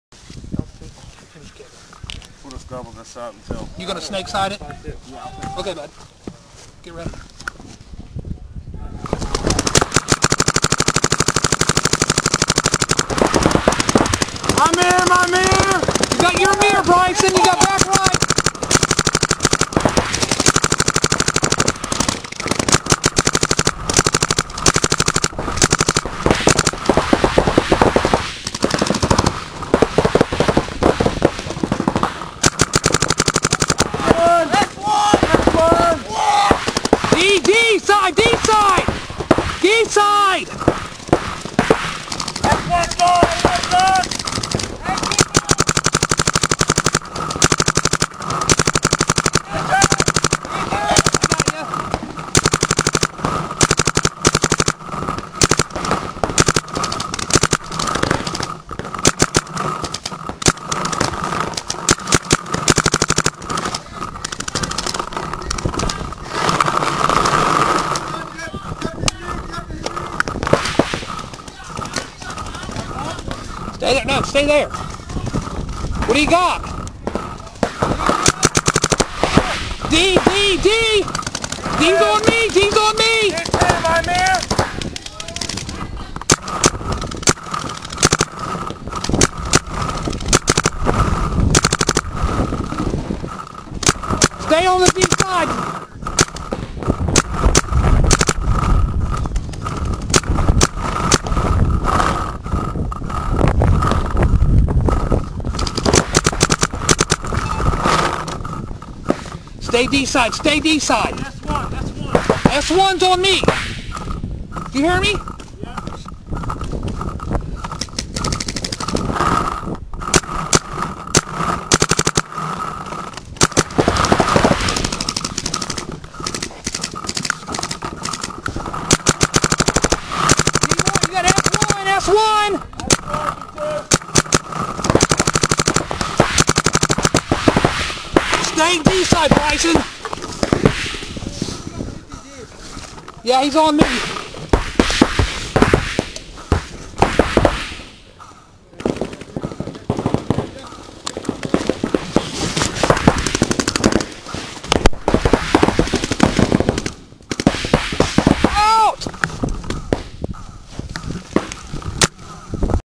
MAO shootyball GRIND...
New 2014 PSP field on that MAO layout out Paintball Country...